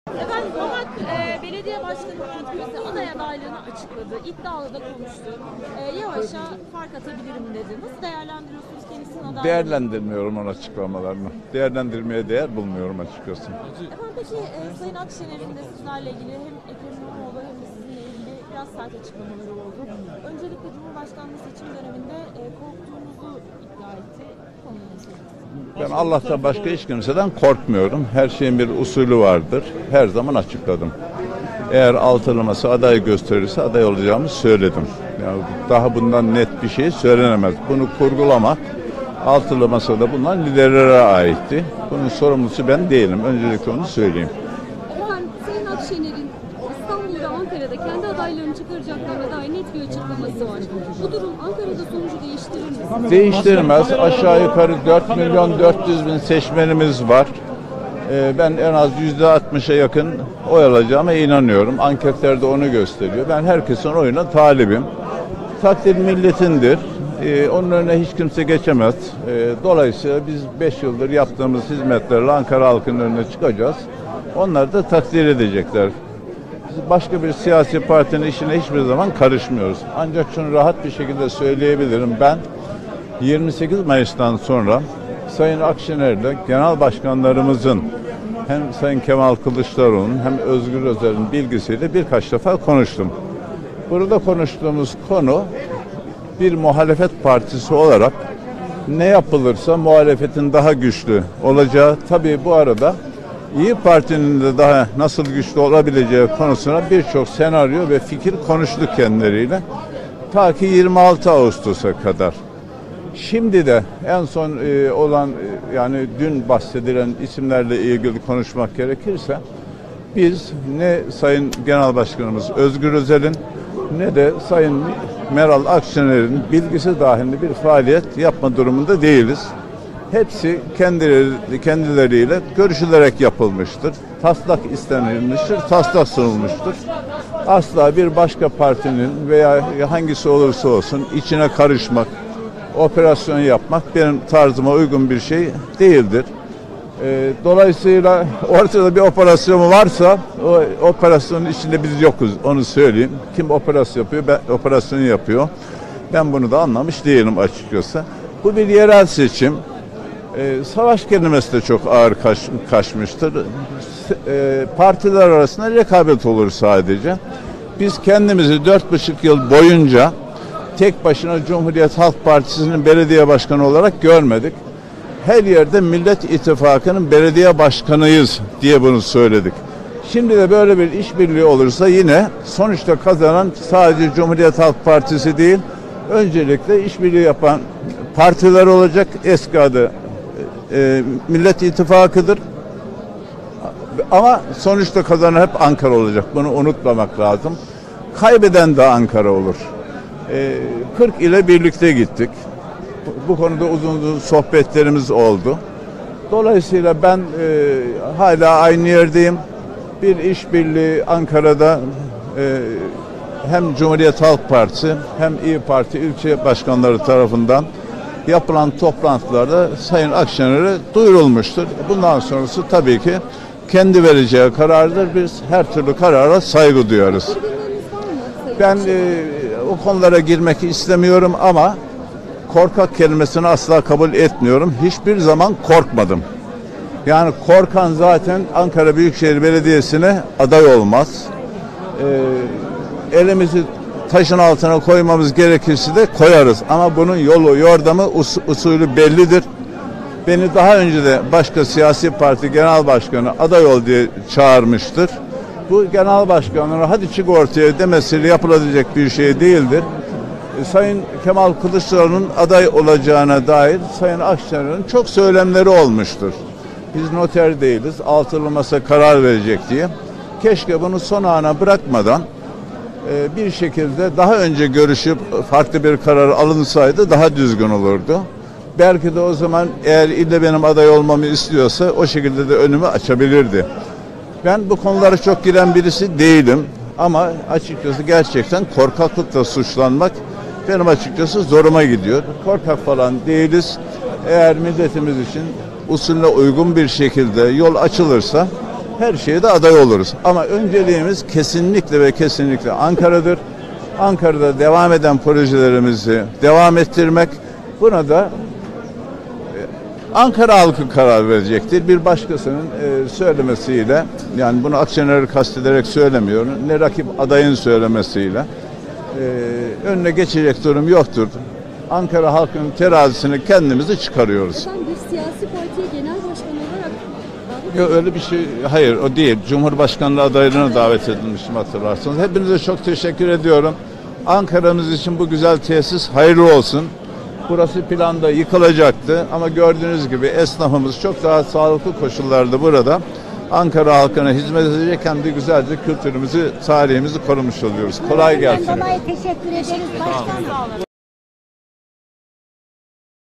FLAŞ Meral Akşener 'korkaklıkla' suçlamıştı! Mansur Yavaş sessizliğini bozdu...
Önceki gün İBB Başkanı Ekrem İmamoğlu ve ABB Başkanı Mansur Yavaş'ı kastederek "İkisi de korkup, milletin isteğini kabul etmediler" diyen İYİ Parti lideri Meral Akşener'in o sözlerine, Yavaş'tan yanıt geldi. Mansur Yavaş, "Korkaklıkla suçlanmak açıkçası zoruma gidiyor" dedi. ABB Başkanı ayrıca, "İlle benim aday olmamı istiyorsa önümü açabilirdi" diye konuştu.